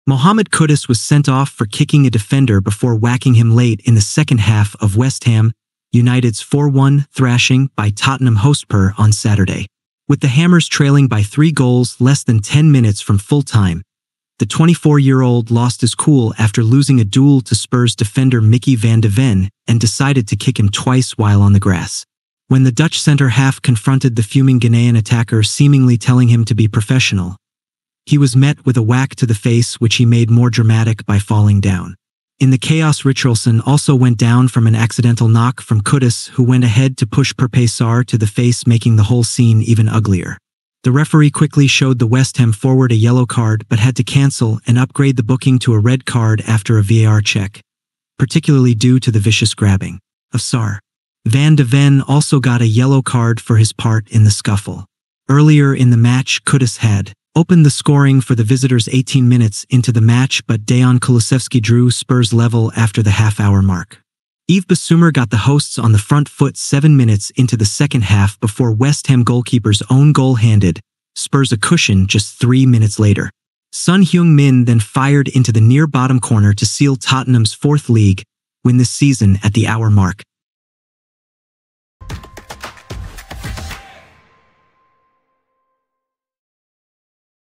Mohammed Kudus Red Card Against Tottenham Hotspur
Mohammed Kudus was sent off for his clashing with Micky van de Ven during West Ham United's 4-1 defeat by Tottenham Hotspur in a Premier League clash on Saturday.